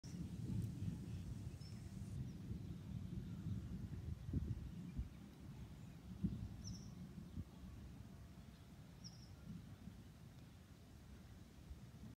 Banno thunderstorm
Today we had a thunderstorm! First one for the year. No tornado though :(. But it was really cool! There also might be another one tomorrow!